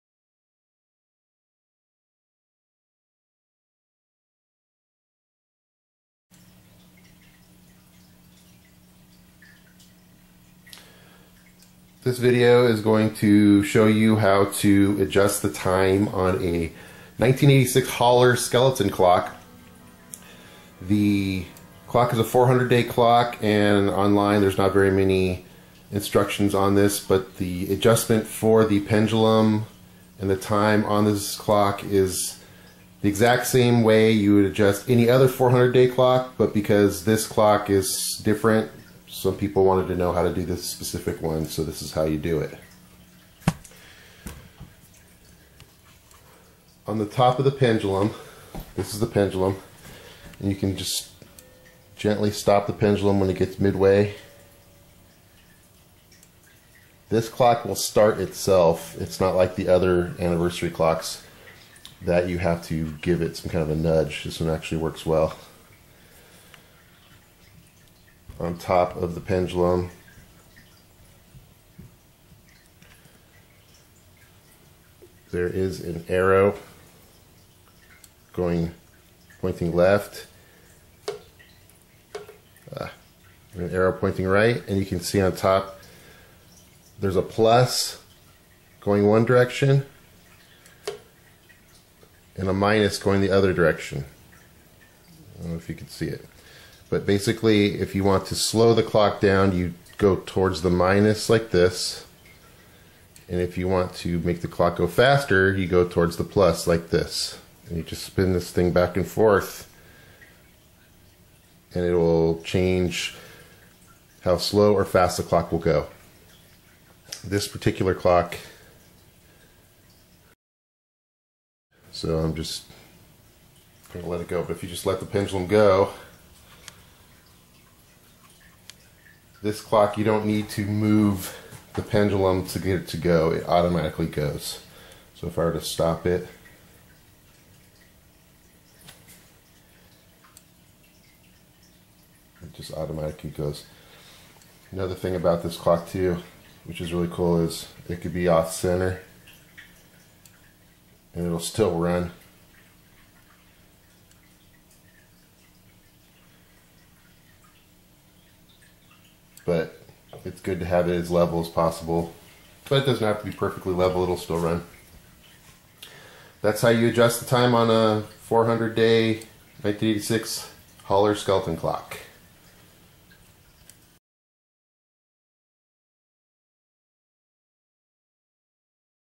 Elgin Sigfried Haller Skeleton Anniversary Clock time adjustment
This video will explain how to adjust the time on a Elgin Sigfried Haller Skeleton Anniversary Clock Germany c. 1986. The time adjustment is the same as any other standard 400 day Anniversary clock.